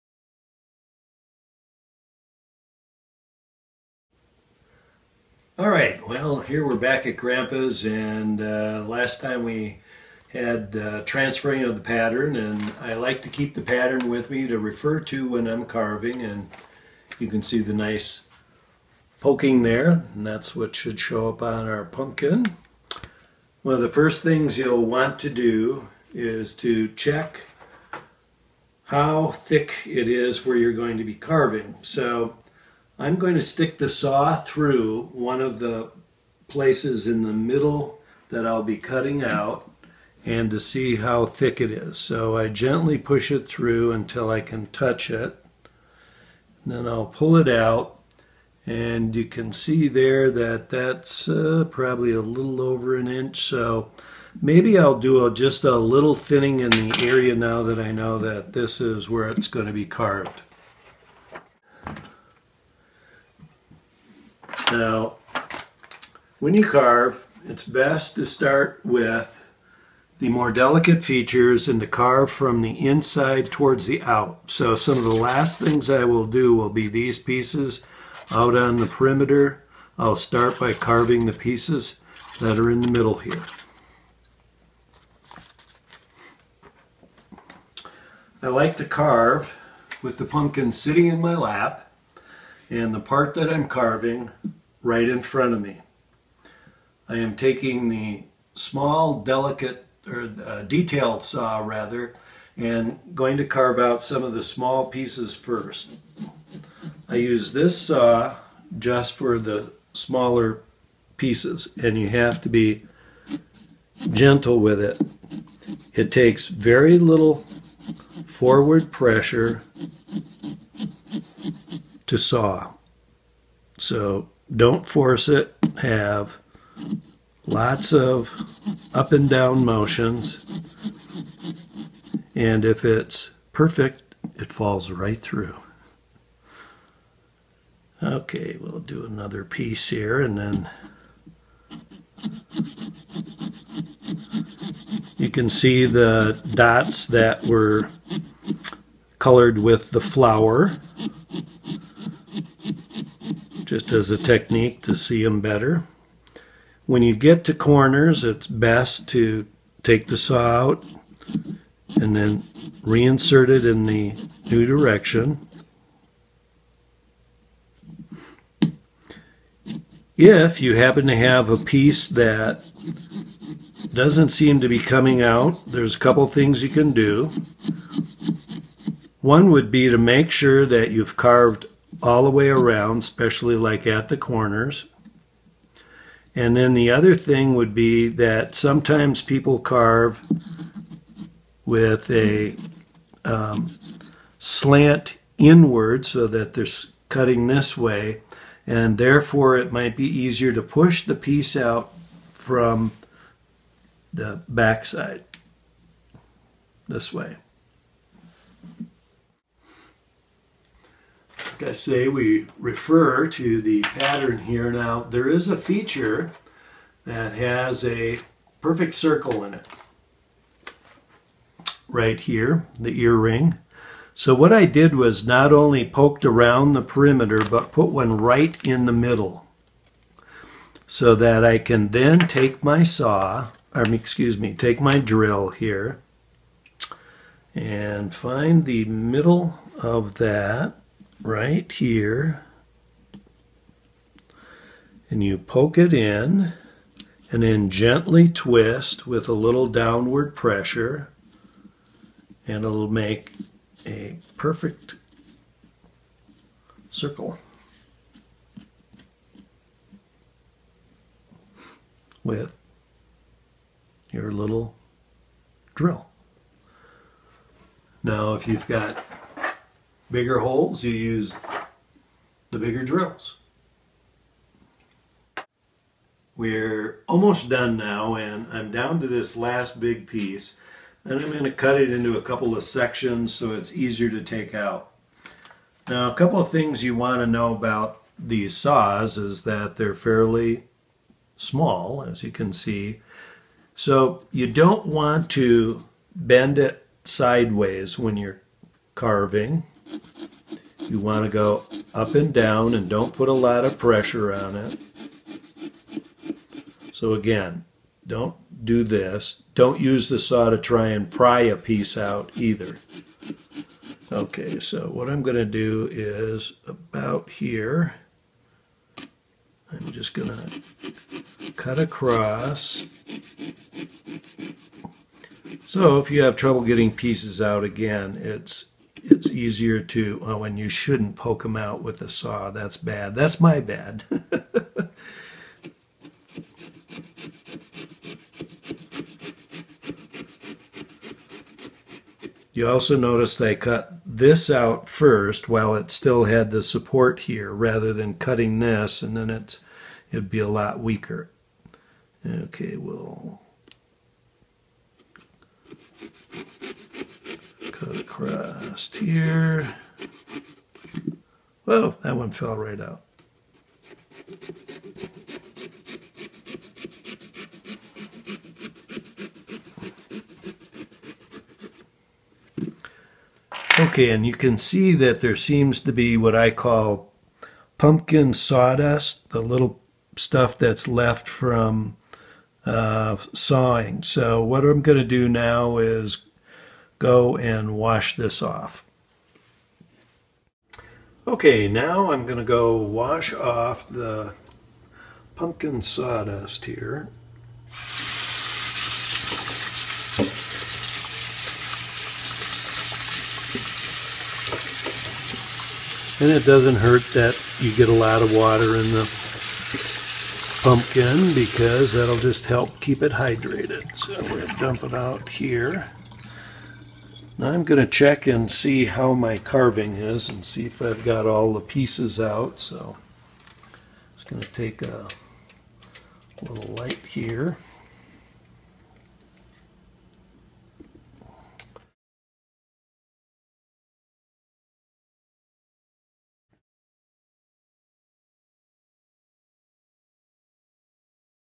Carving Your Pumpkin
Master carver John "Grampa" Bardeen shows you how to carve a Halloween pumpkin like a pro. Grampa Bardeen's Family Pumpkin Carving Sets are available at GrampaBardeen.com and Amazon.com (eligible for Prime shipping).

for more pumpkin carving tips and tricks!